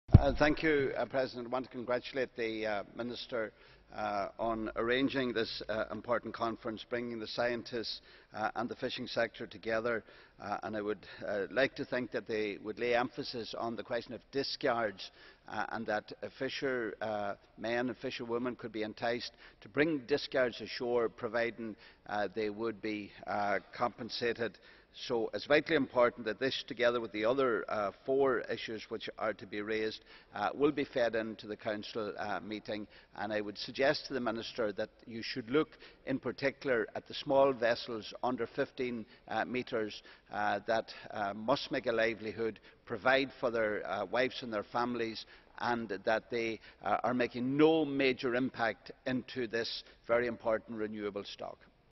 Pat the Cope Gallagher on Question Time (Council)
Speech by Pat the Cope Gallagher MEP (ALDE ADLE) on :
Question Time (Council)

Gallagher_100922_181744_mesp_en